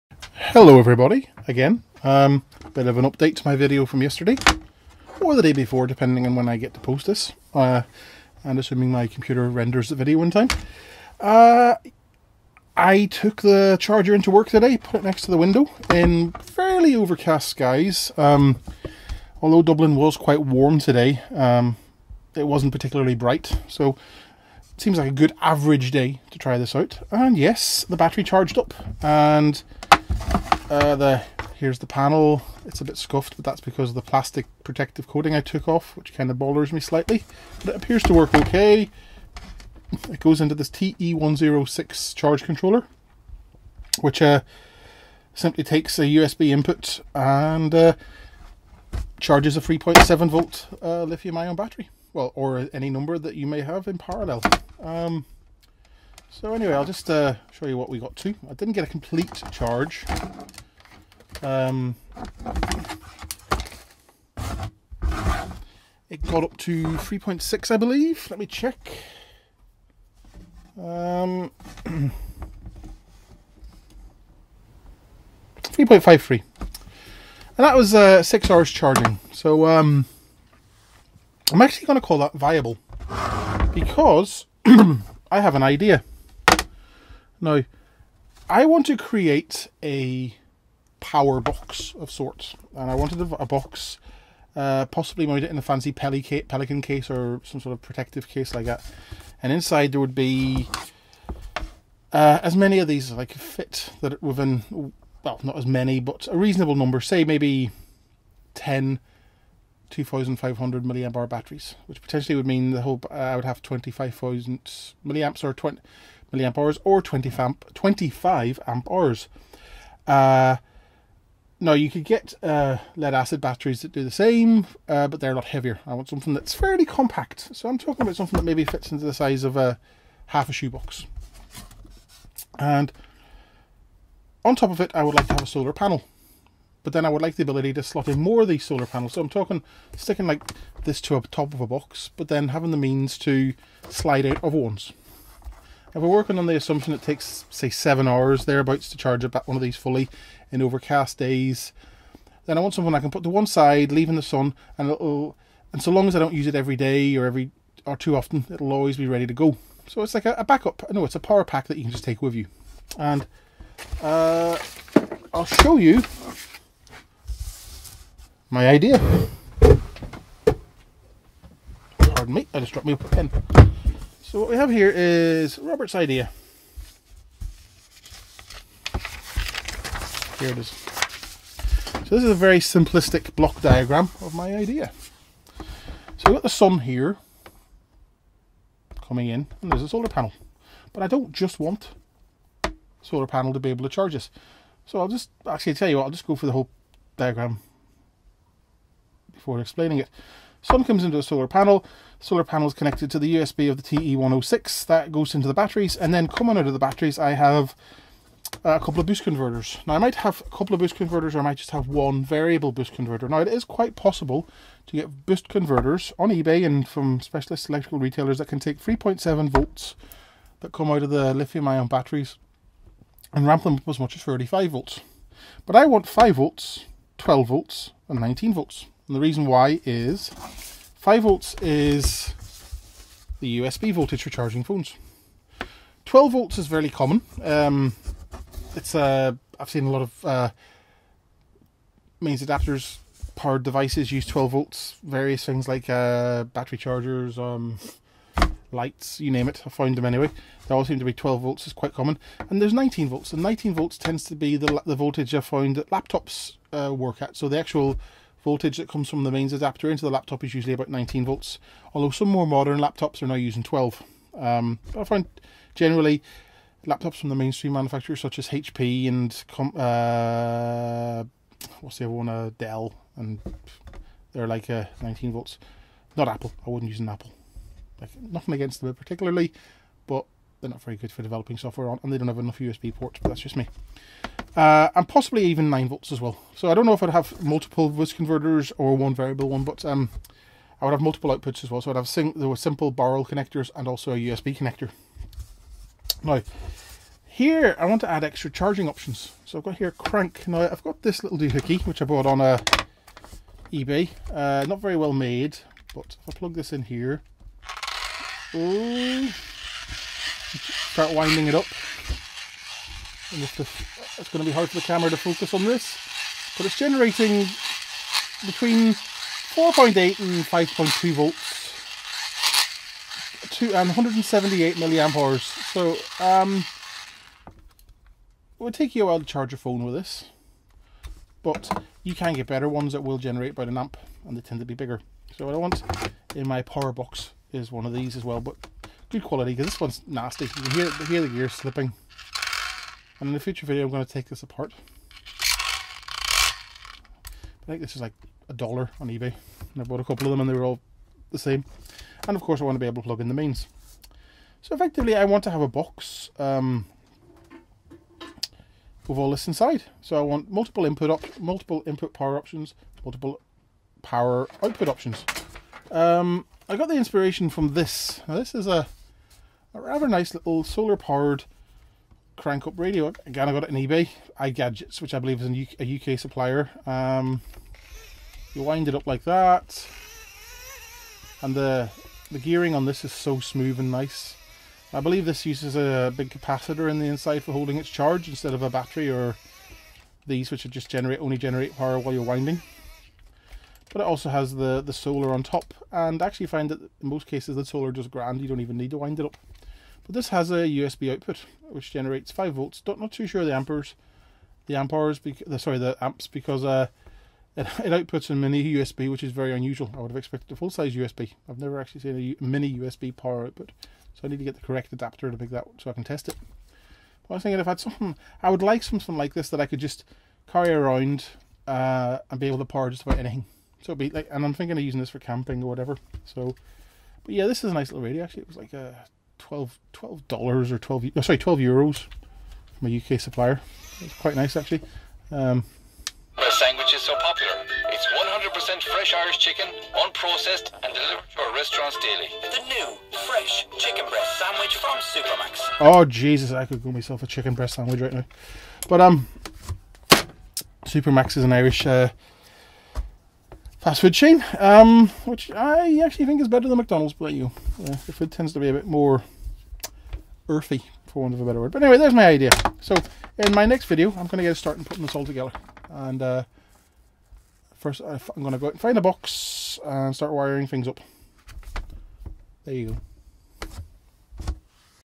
A DIY Solar USB Power Bank -Part 2
The charger prototype worked, so where next?